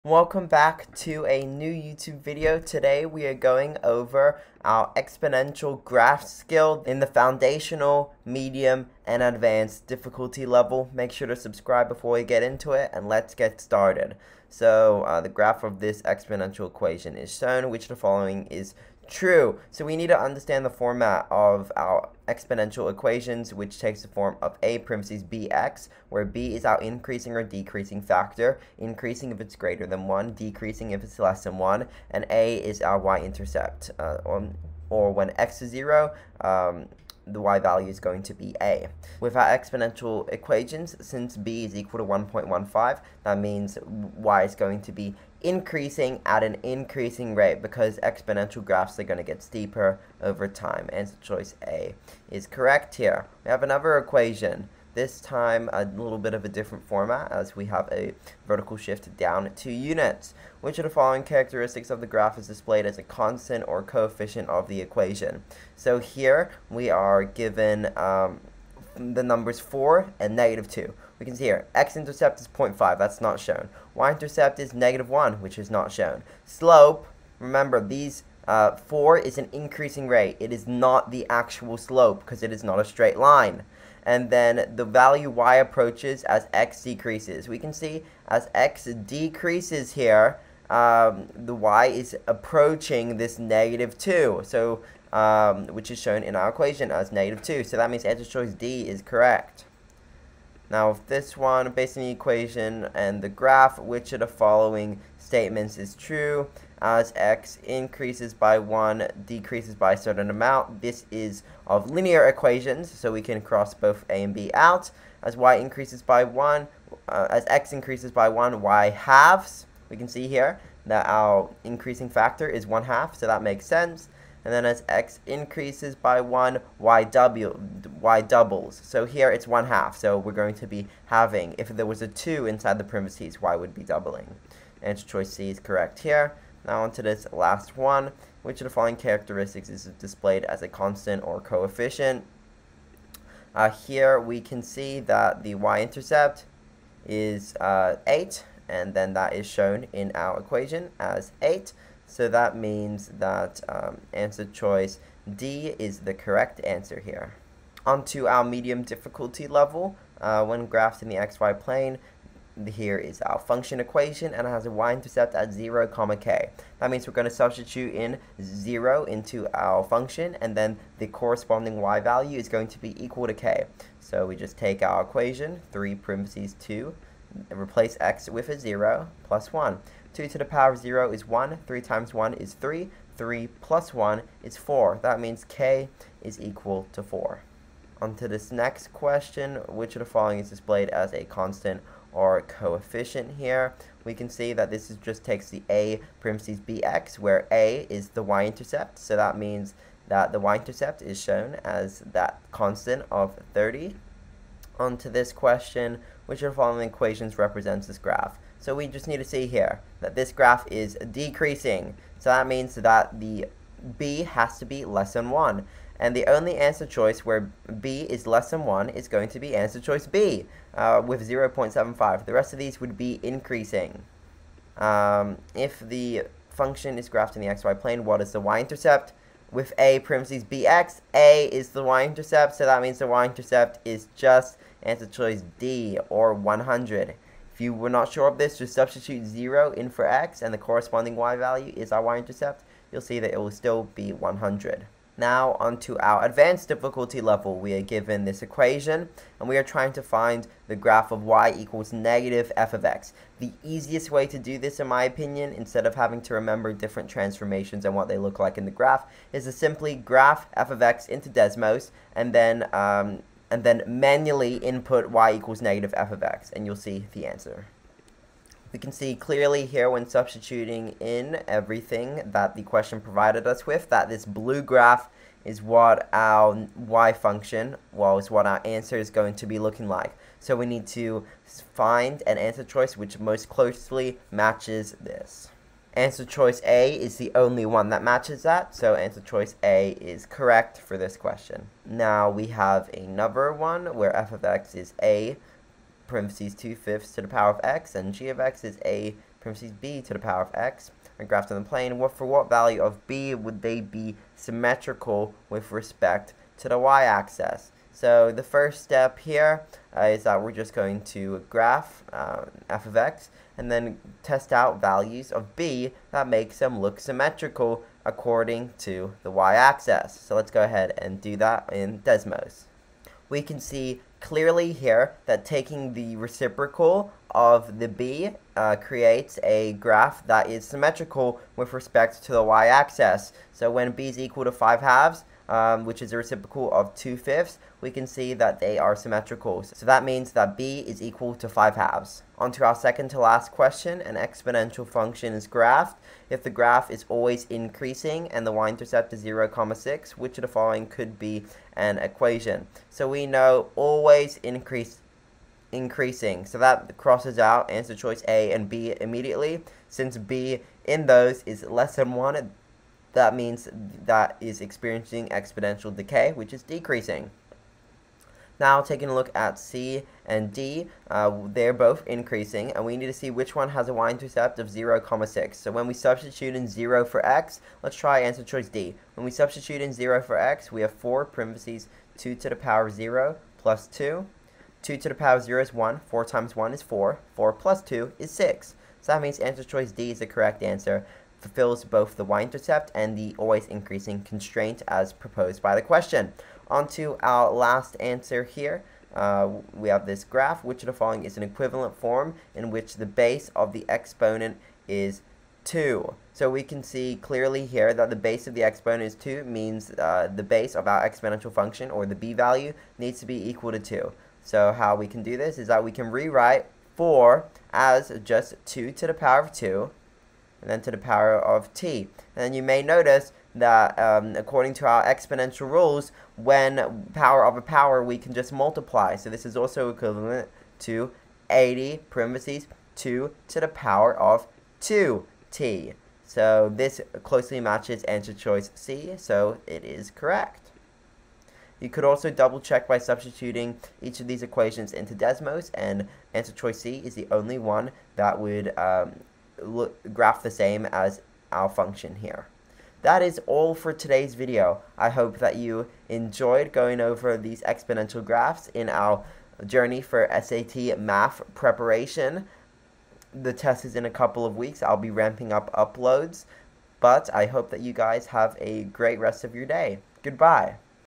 SAT Math: Exponential Graphs
In this video, I cover 12 questions of various difficulty levels related to the exponential graphs skill inside of Khan Academy's SAT Math prep course. Have a fantastic day!